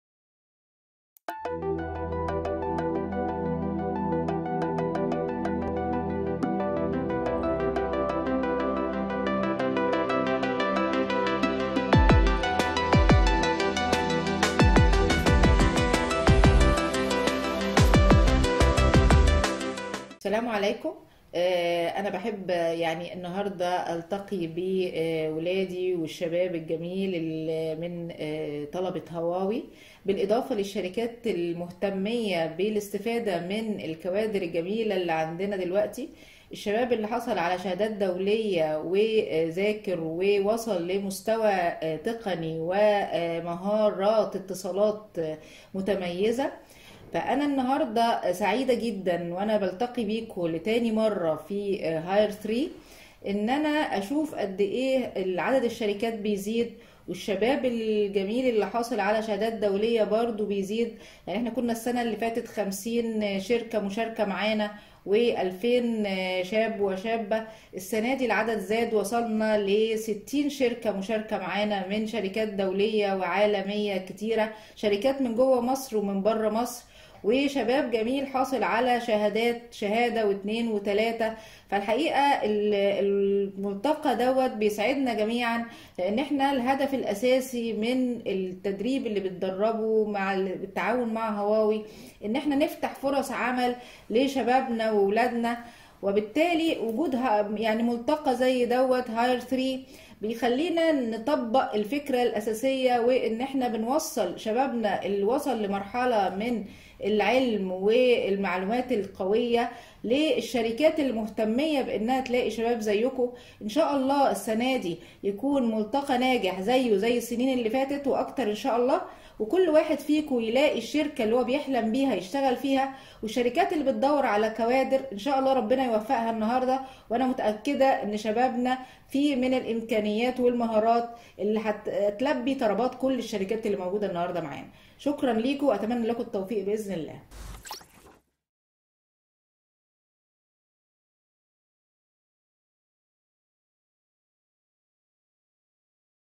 Dr. Iman Ashour, National Telecommunication Institute (NTI) President - HiRE 3 opening speech
HiRE3 is designed to introduce well-trained & internationally certified talents in the different ICT technologies, graduated from Huawei iTB Program (ICT Talent Bank), to the main pillars of the ICT market.

Thus strongly enabling digital transformation momentum and serving as a complete ecosystem for an open and wide-ranging employment and career development opportunities for the Egyptian youth.

HiRE3 will be an online event organized by Huawei iTB Program and sponsored by its main governmental sponsors, Ministry of Communication & Information Technology (MCIT) & its affiliate National Telecommunication Institute (NTI), in addition to the Ministry of Man Power (MOMP).

The expo will accommodate a wide spectrum of diverse entities in the ICT field, such as: international/national corporates, governmental ministries, non-governmental organizations, business incubators, startup business accelerators, highly contributing freelancers and influencers, all under one roof to engage with highly competitive, iTB certified talents.

HiRE3 will include: executive keynotes and panel discussion, professional technology & career development sessions by HR professionals, creative entrepreneurs and high quality freelancers, and booths for recruiter companies to conduct the screening & interviewing with the talents.